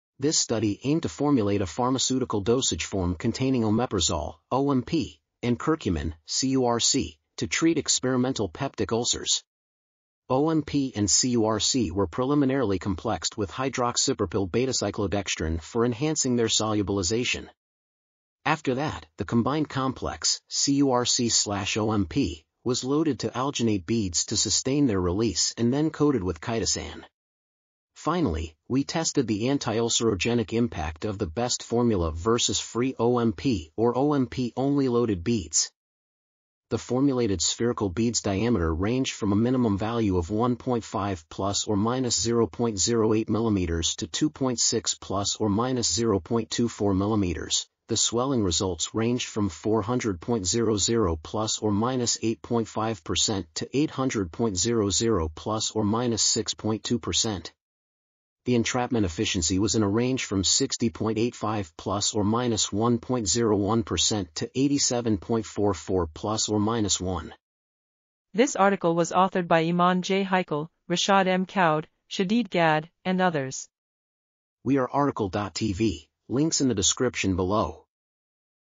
Design and Optimization of Omeprazole-Curcumin-Loaded Hydrogel Beads Coated with Chit... | RTCL.TV
### Keywords ###

### Article Attribution ###
Title: Design and Optimization of Omeprazole-Curcumin-Loaded Hydrogel Beads Coated with Chitosan for Treating Peptic Ulcers
Authors: Eman J. Heikal, Rashad M. Kaoud, Shadeed Gad, Hatem I. Mokhtar, Afaf A. Aldahish, Wafa Ali Alzlaiq, Sawsan A. Zaitone, Yasser M. Moustafa ,and Taha M. Hammady

### Video Timestamps ###
0:00:00 - Summary
0:01:08 - Title
0:01:14 - Outro
0:01:18 - End